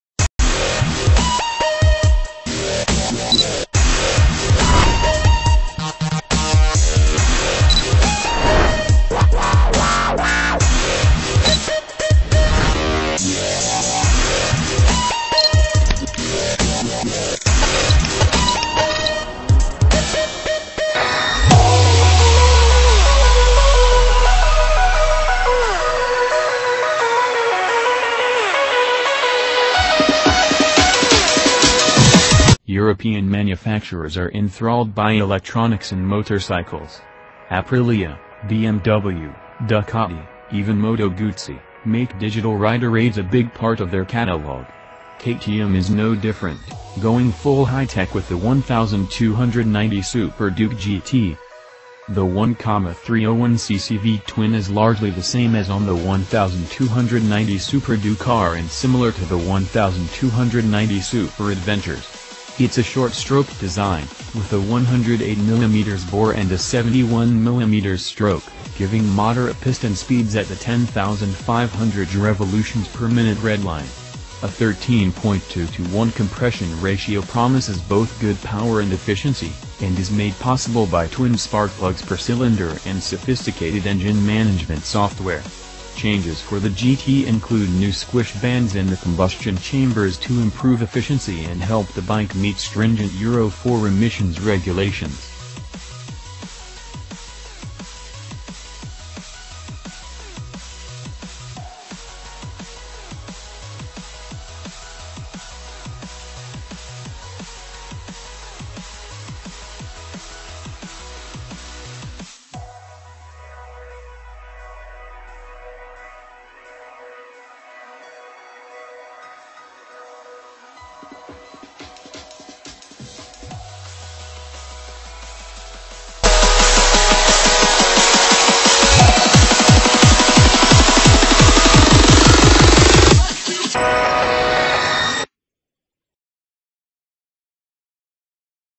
MUST WATCH!!KTM 1290 Super Duke GT Design Review
MUST WATCH!!KTM 1290 Super Duke GT Design Review - European manufacturers are enthralled by electronics in motorcycles. Aprilia, BMW, Ducati—even Moto Guzzi—make digital rider aids a big part of their catalog. KTM is no different, going full high tech with the 1290 Super Duke GT

$19,999